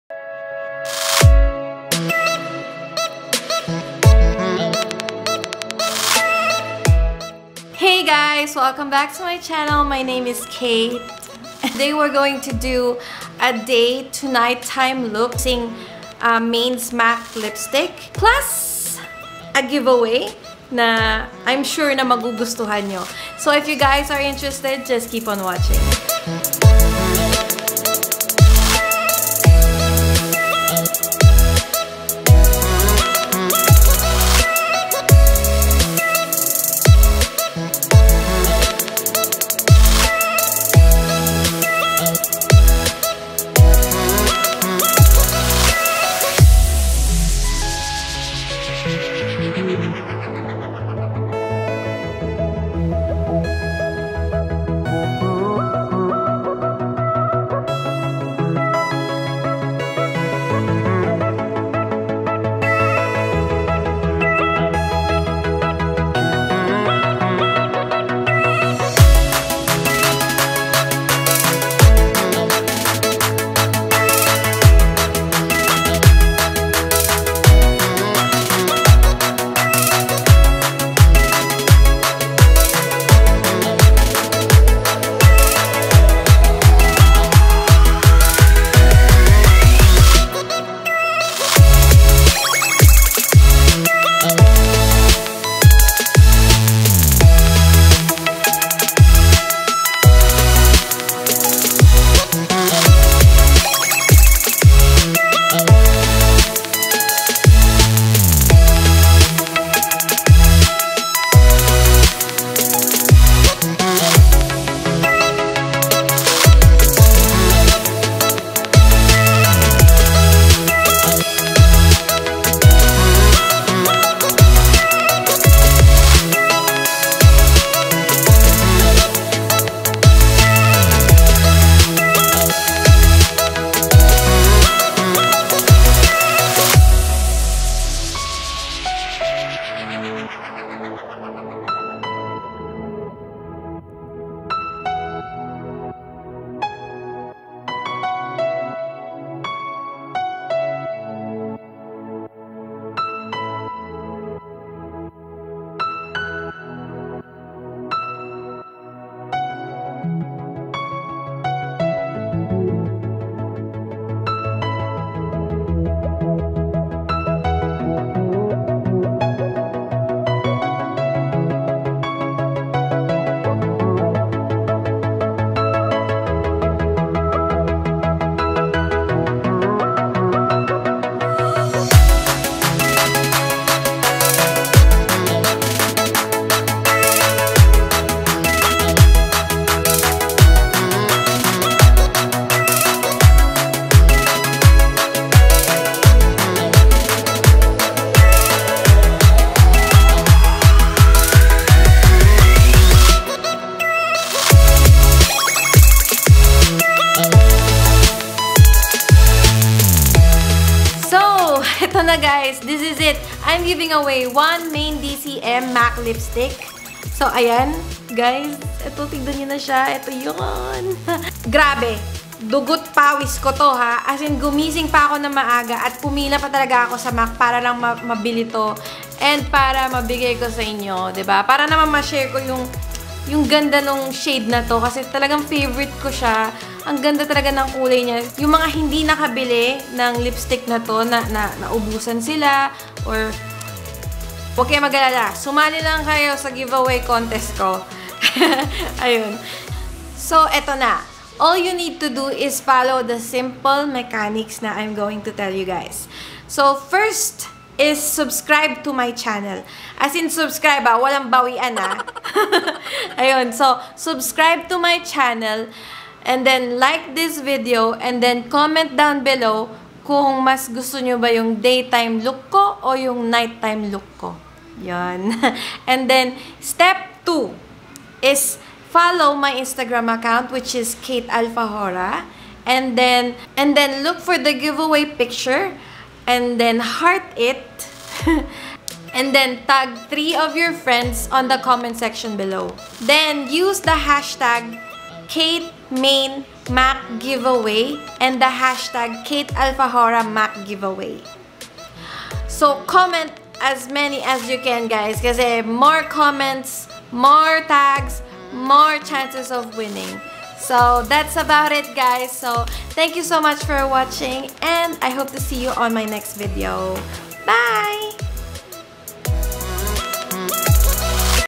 Maine Mendoza Mac Lipstick Day to Night time look | @MAINEDCM GIVE AWAY
Studio shoot at @bellestudioph Follow them on IG

Products used:
Smashbox Primer Pore Minimizing
MAC Prolong wear concealer NC30
MAC Studio Foundation NC25
RCMA no color Powder
Colourette browfessional Mocha
Skin Potions Dream Cream Scarlett
Milani Blush Bella Bellini (eyes)
Careline Graph Ink Liner
MAC blush Harmony
MAC Mineralize blush Love Joy
MAC Maker @MAINEDCM
Morphe 350 palette (for the night time look)

Winner will be announced on my IG account on October 16, 2018

Good Luck!